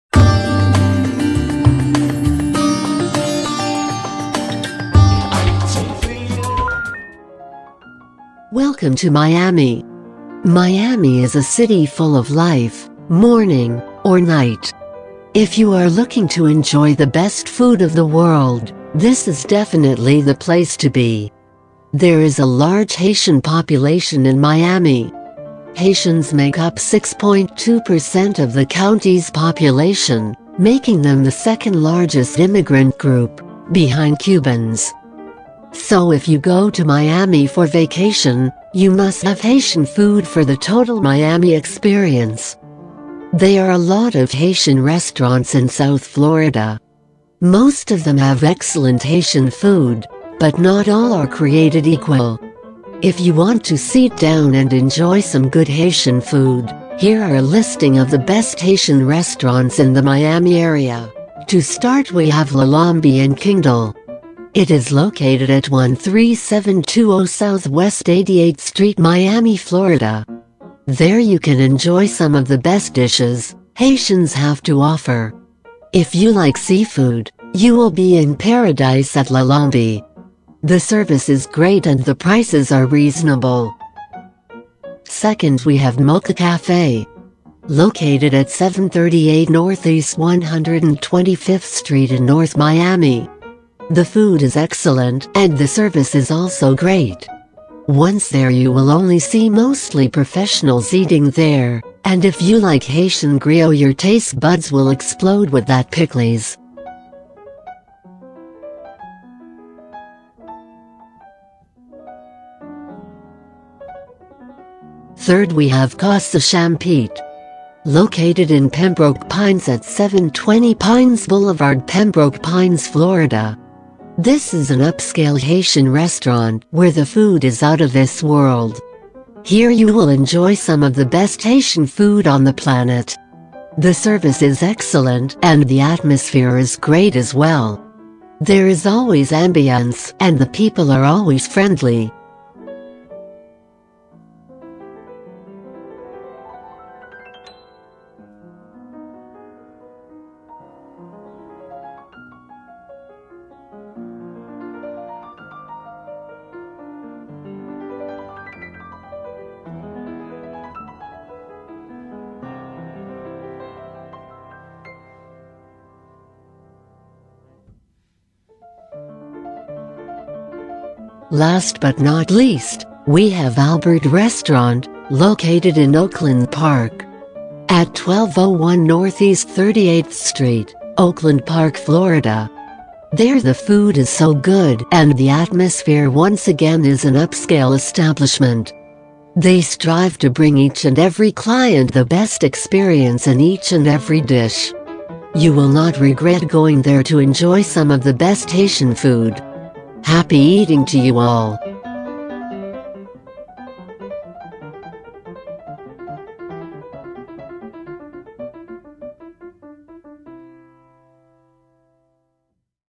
The Best Haitian Restaurants In South Florida
For us, food offers one of the most enjoyable contexts through which to understand a place. As we seek out certain types of dishes, we find ourselves in new experiences of all sorts.
Of all the fruits of the sea you can find in Haiti, conch seems to be among the most distinct to appear on restaurant menus. You can usually find it grilled (see below) or in a tomato-based creole sauce. Conch is a must-try if seafood is your thing.